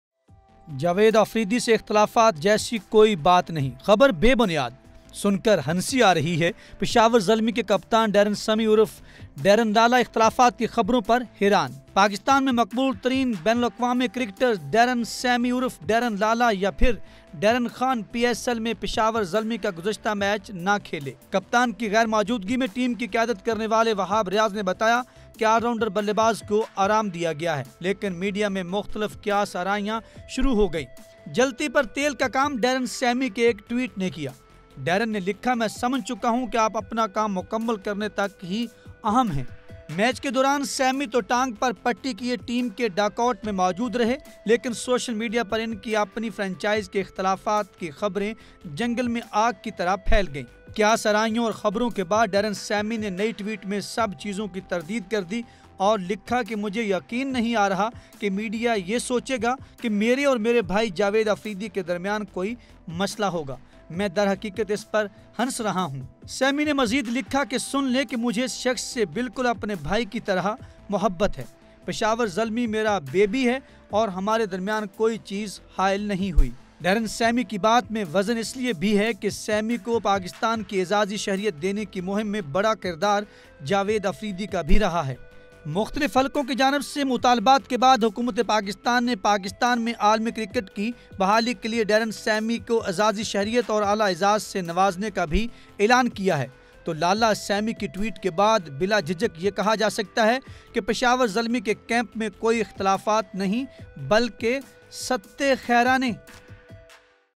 Sammy issues a clarification on rumors of conflict with Javed Afridi
Darren Sammy issues a clarification on rumors of conflict with Javed Afridi